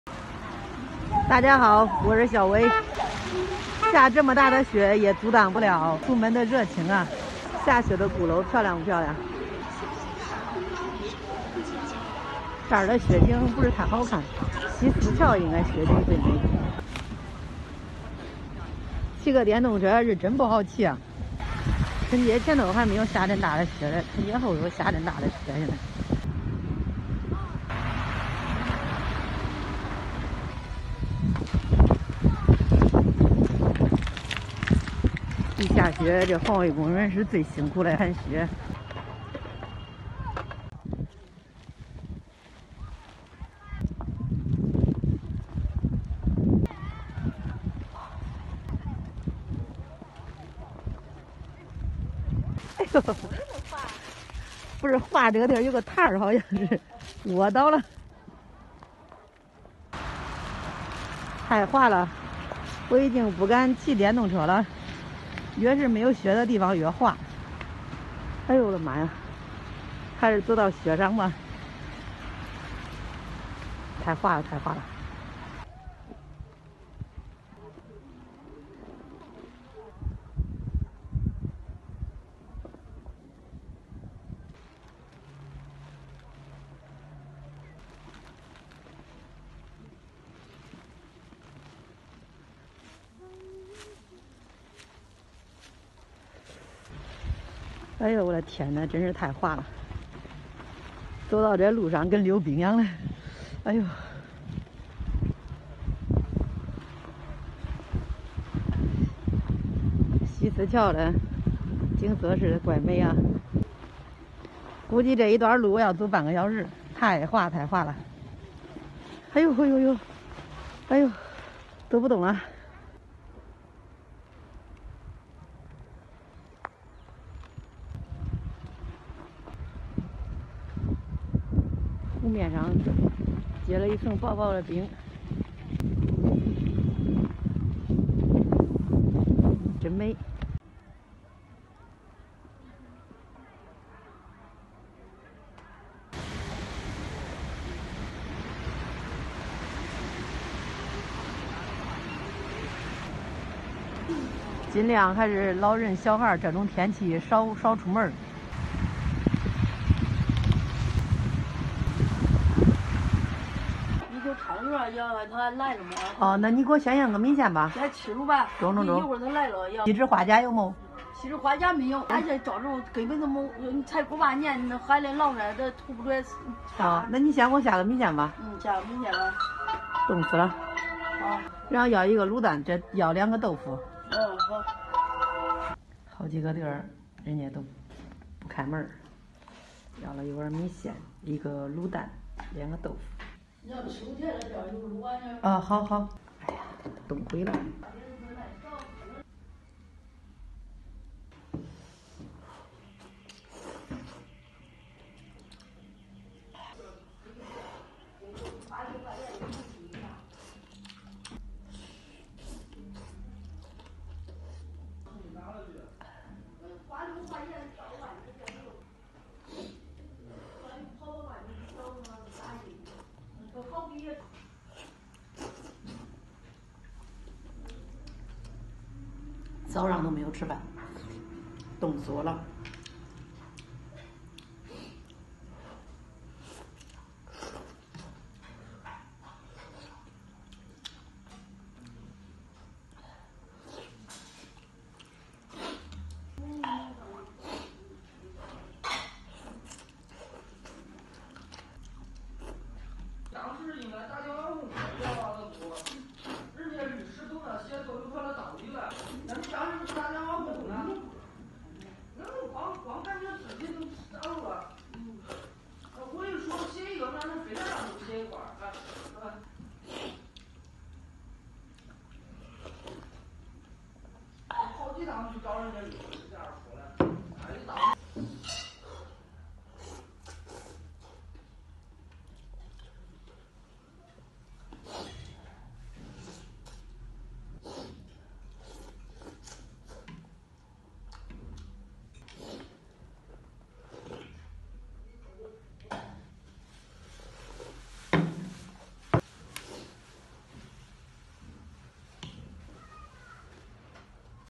开封下大雪好多饭店关张，小微转一圈又冷又饿，吃的啥连汤都不剩
"我们的目的是分享美味食品的制作过程，所有食材的获取和使用都符合道德和法律原则。" 开封下大雪好多饭店关张，小微转一圈又冷又饿，吃的啥连汤都不剩